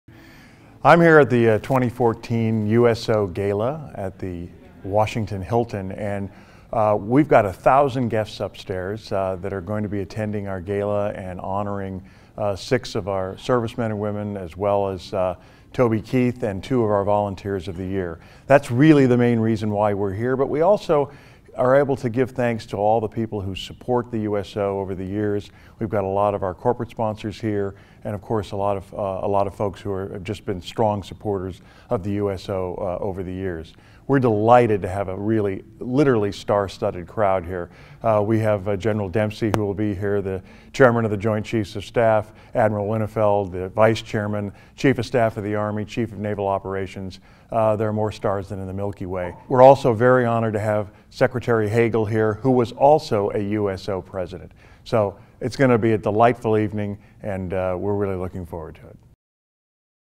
USO President and CEO Dr. J.D. Crouch II at the 2014 USO Gala
USO President and CEO Dr. J.D. Crouch II shares what attendees have to look forward to at this year's gala.